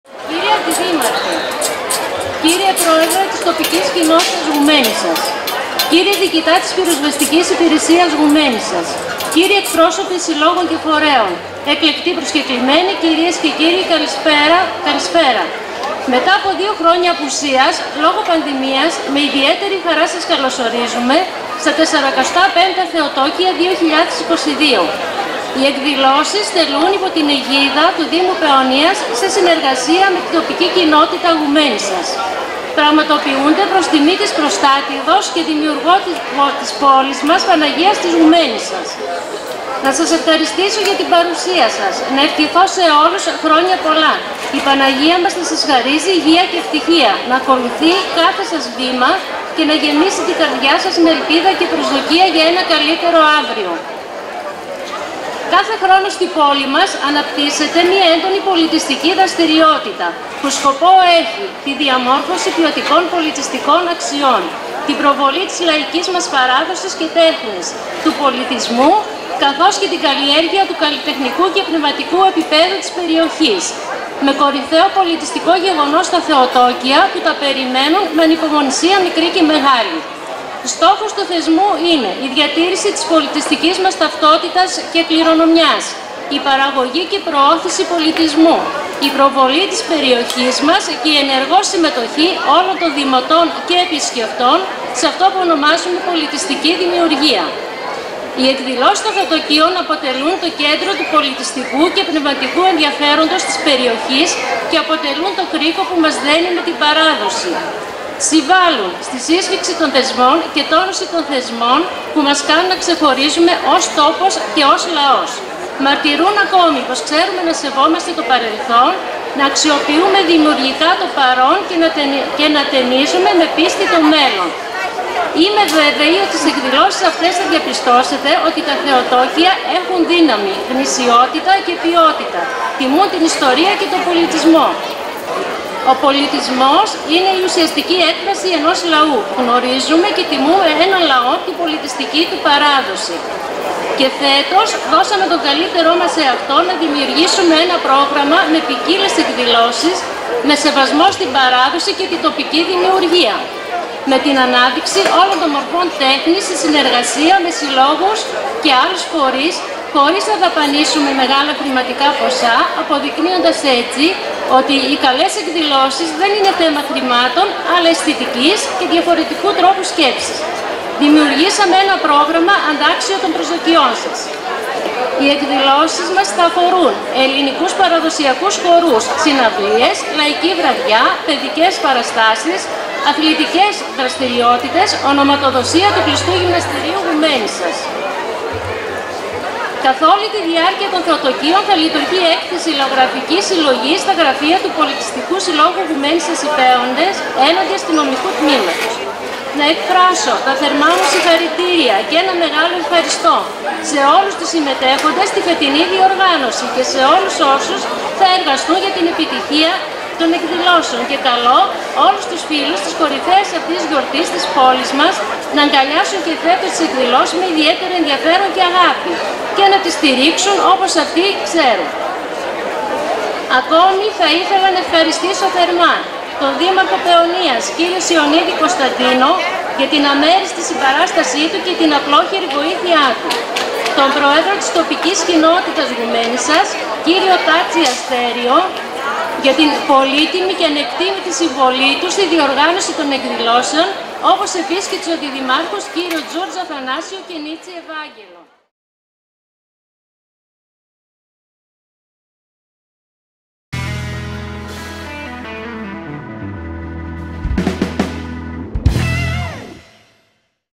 Καλωσόρισμα από την Μαρία Παλάνη στα 45α Θεοτόκια "2022"
Πέμπτη 11/8/2022
Μακεδονική  Βραδιά
Ας Κρατήσουν οι Χοροί
45α Θεοτόκια Γουμένισσας
Π.Σ. Γουμένισσας Οι Παίονες

Μαρία Παλάνη Αντιδήμαρχος
Δημοτικής Ενότητας Γουμένισσας